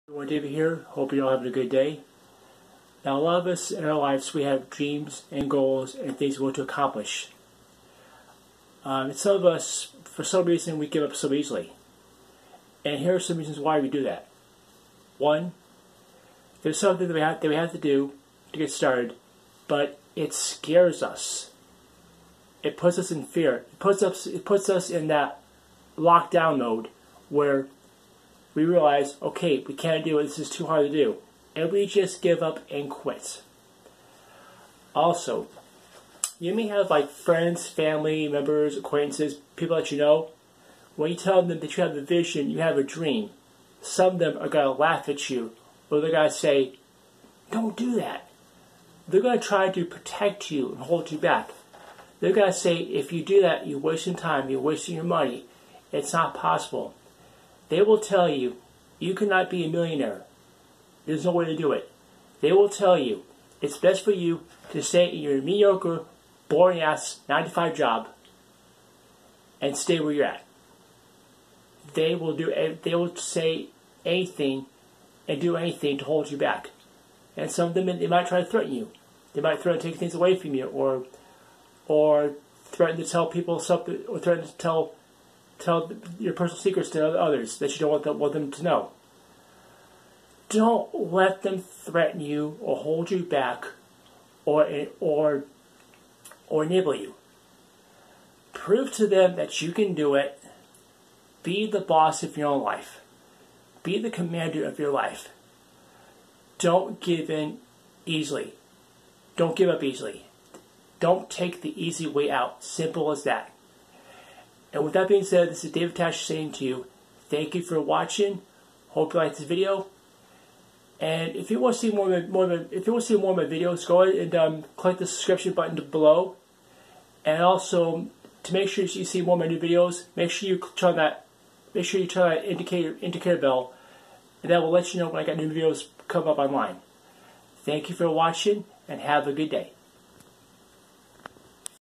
Don"t Take The Easy Way Out
Here I talk about why so many people give up on their dreams and why it's important to not give up on them. It's important for all of us to work for our dreams and make them a reality.